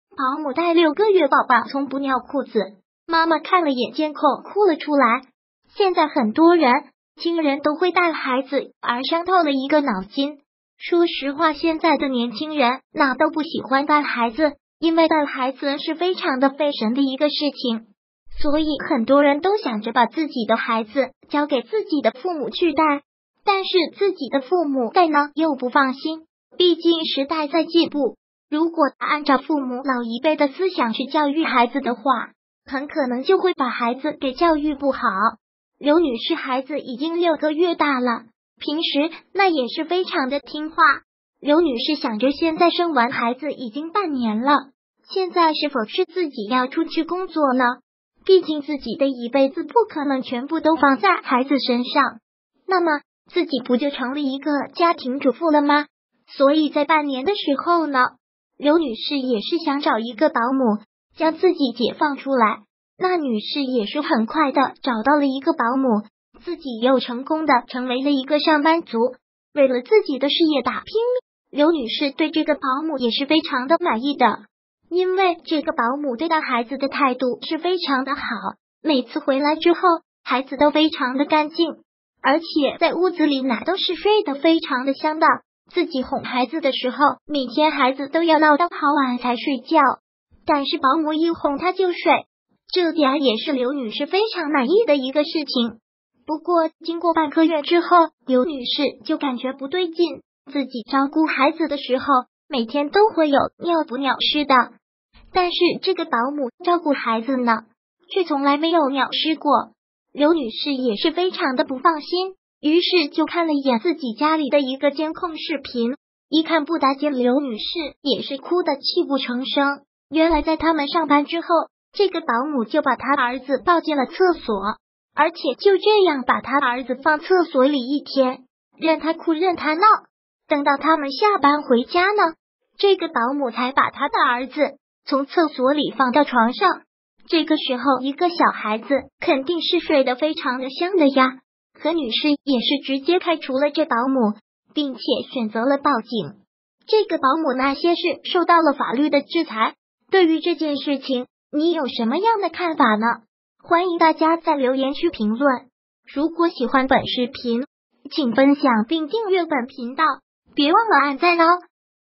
保姆帶6個月大的寶寶從不尿褲子，媽媽看了眼監控哭了出來！
保姆帶6個月大的寶寶從不尿褲子，媽媽看了眼監控哭了出來！育兒保姆帶6個月大的寶寶從不尿褲子，媽媽看了眼監控哭了出來！育兒